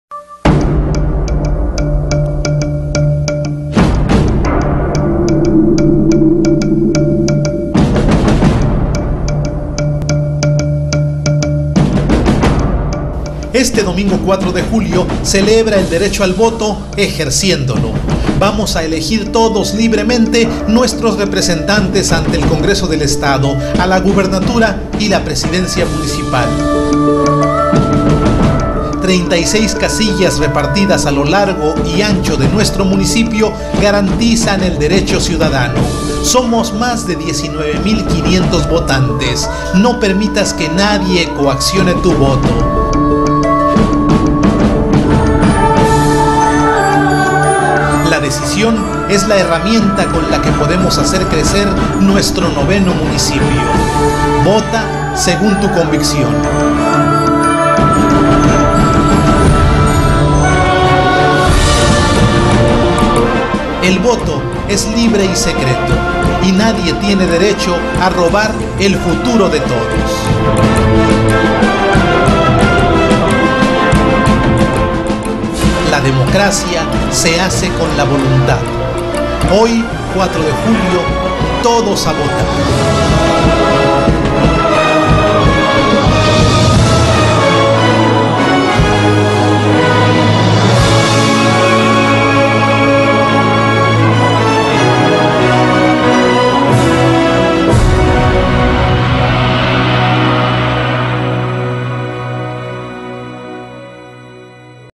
4 DE JULIO, EJERCE TU VOTO.mpg
Una invitación al voto de Tulum en Red e Informativo Tulum que hoy transmite lo más importante de la semana además con una cobertura especial de las elecciones en Tulum